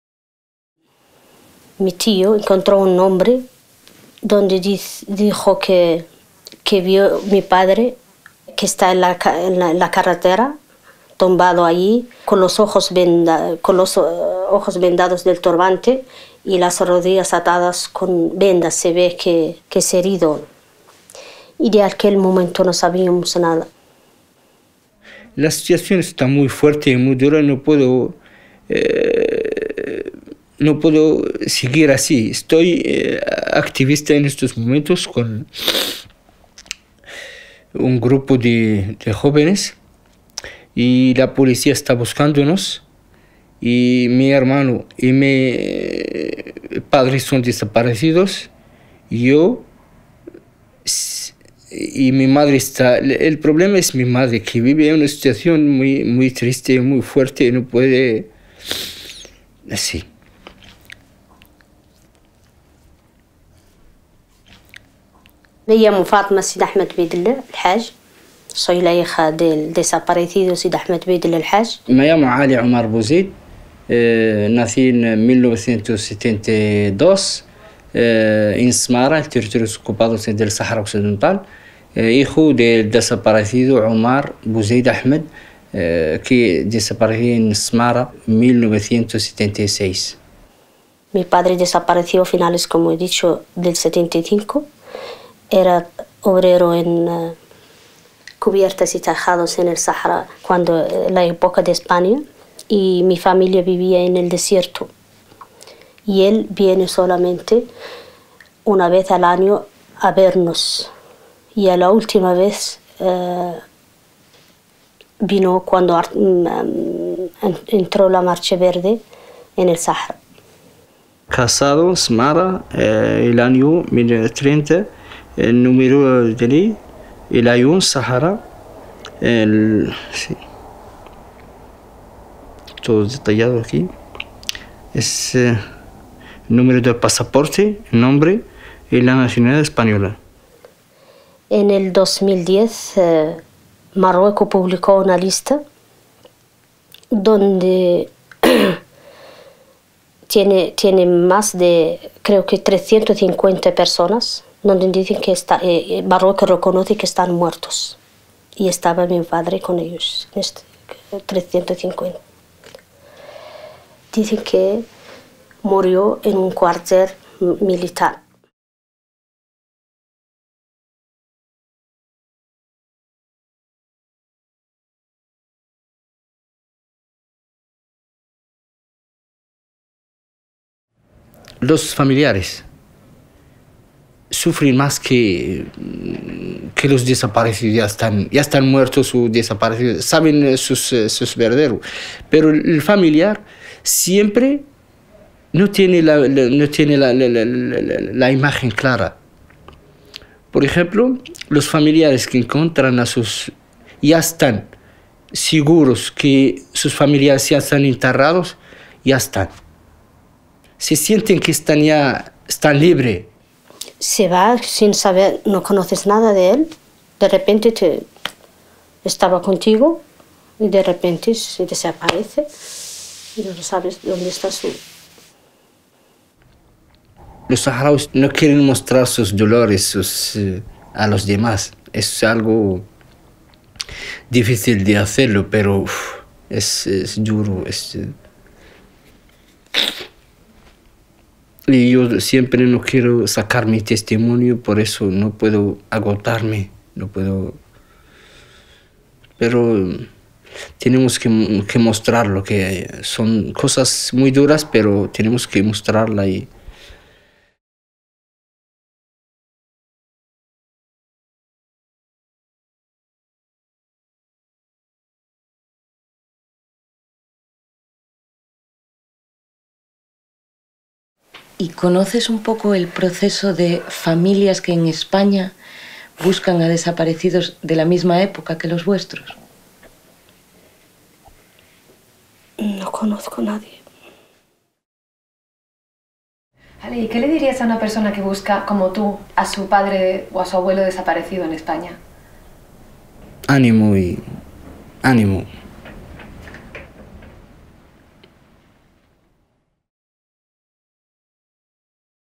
Buscar a un padre
¿Sabías que hasta el año 1976 España tuvo una provincia más que las que conocemos actualmente? No te preocupes si no: a mucha gente le pasa. Se trataba del Sáhara Occidental, colonizado desde finales del siglo XIX, y cuyo abandono se de el mismo día de la muerte de Franco. Esta es la historia de la provincia 53, y de cómo la partida de España dejó irresuelta la historia del que sigue siendo el único territorio pendiente de descolonización de África. Una historia que tiene todo que ver con la dictadura y con la transición, y que nuestra memoria democrática no puede esquivar.